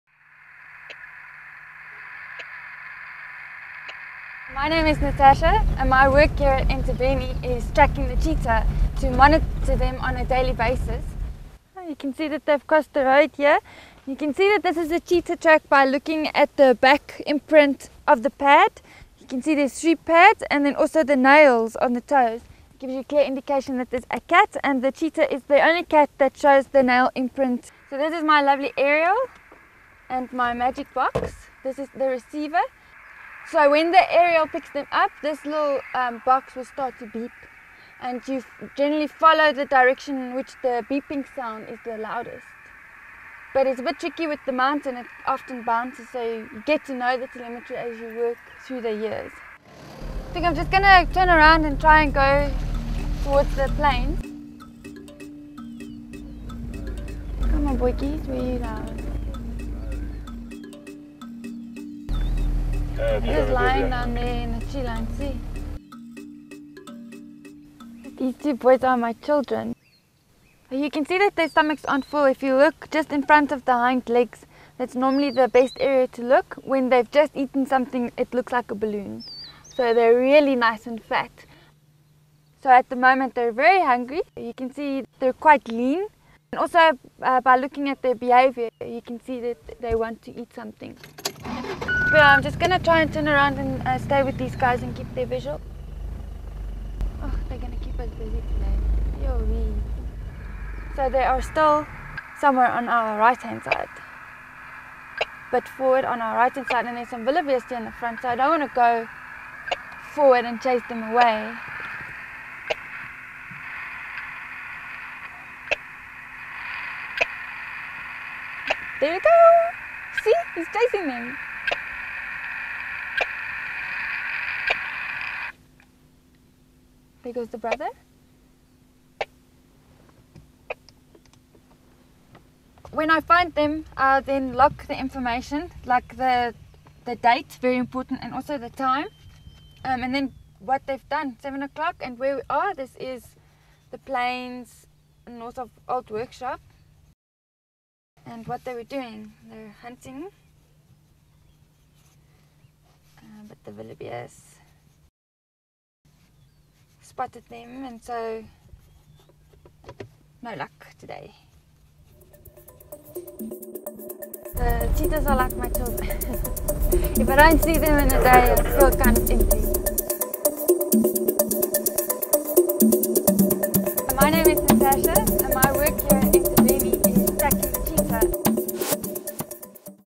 Cheetah Radio
We follow a day in the life of Natasha Nienaber, a Game Management Researcher at Entabeni Private Game Reserve. It is her task to track and observe the Cheetah of the reserve and log any information relating to their eating habits. On this day we experience an exciting encounter of hers with the Cheetah.